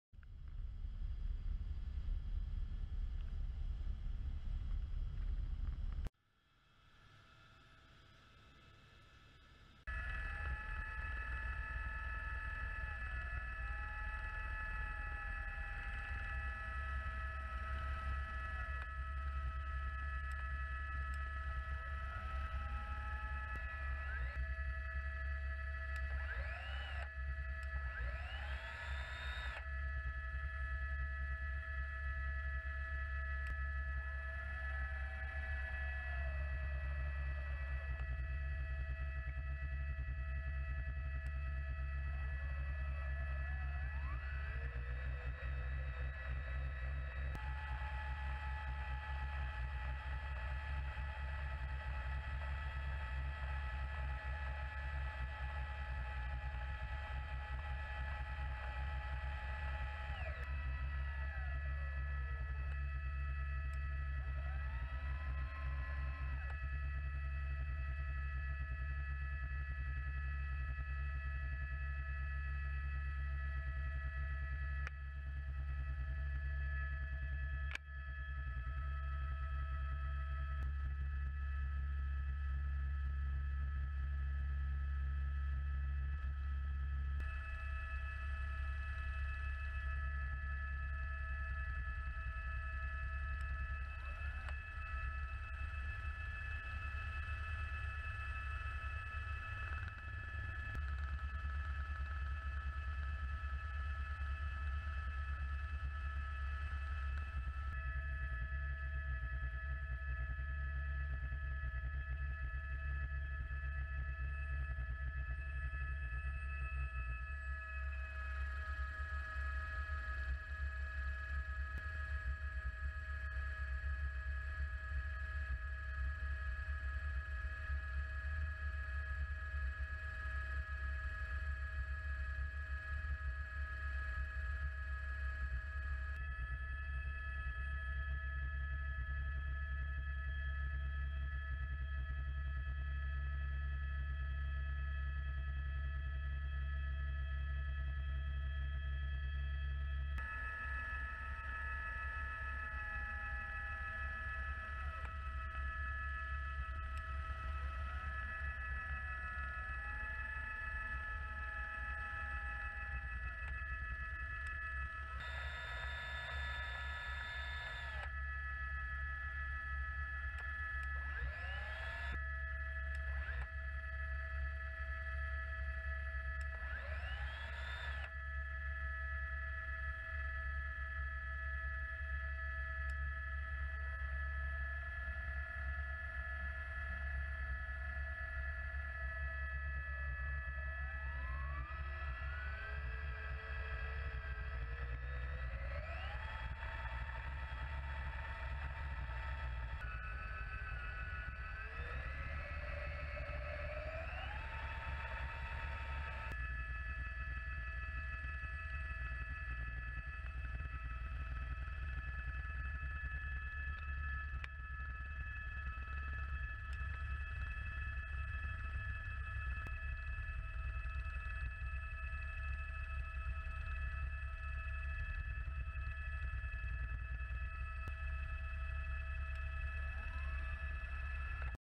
02/03/2019. Rescatada una esquiadora en el Pico del Lago.
El Grupo de Rescate de Protección Civil ha auxiliado a una mujer de 40 años que resultó herida cuando practicaba esquí de travesía en el Pico del Lago, situado en la localidad leonesa de Maraña. Más información